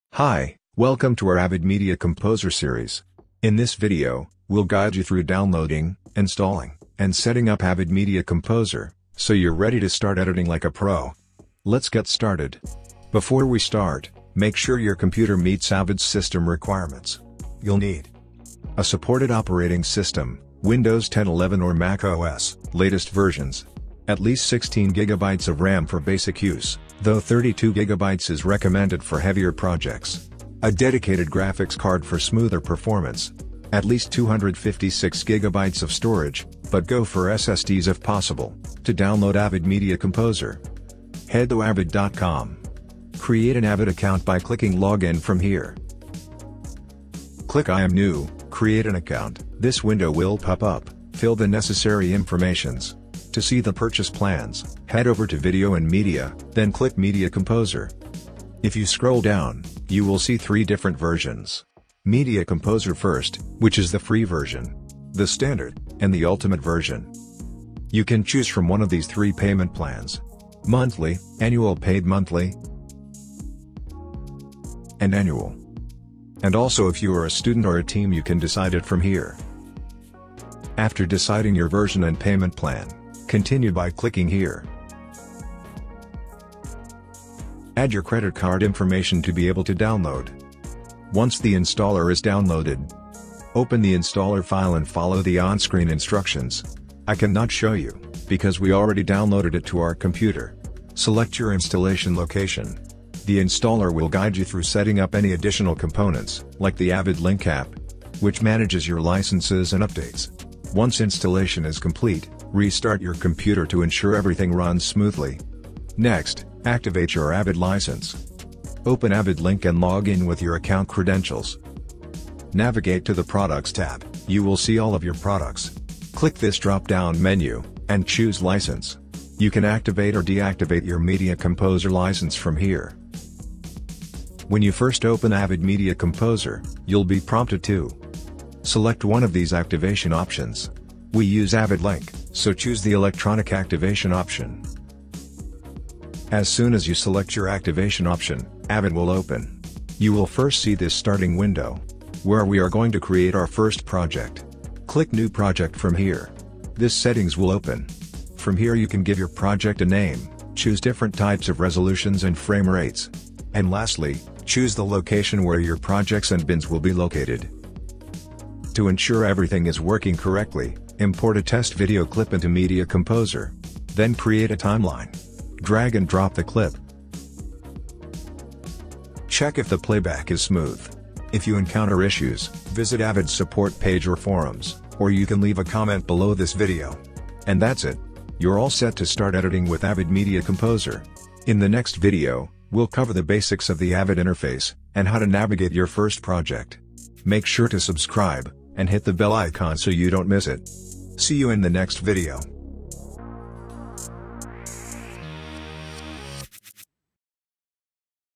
LESSON 1 / INSTALLING & SETTING UP - AVID MEDIA COMPOSER
Welcome to the first episode of our Avid Media Composer series! 🚀 In this video, we’ll show you how to download, install, and set up Avid Media Composer, the industry-standard video editing software trusted by filmmakers and TV professionals worldwide.

Whether you’re new to video editing or looking to switch to Avid, this step-by-step guide will help you hit the ground running.

Here’s what you’ll learn:
✅ System requirements for Avid Media Composer
✅ How to download and install the software
✅ Activating your Avid license
✅ Initial setup and workspace configuration

This video is the starting point of our Avid Media Composer series, where we’ll take you from beginner to pro, one step at a time. In the next episode, we’ll explore the Avid interface and teach you how to navigate your first project.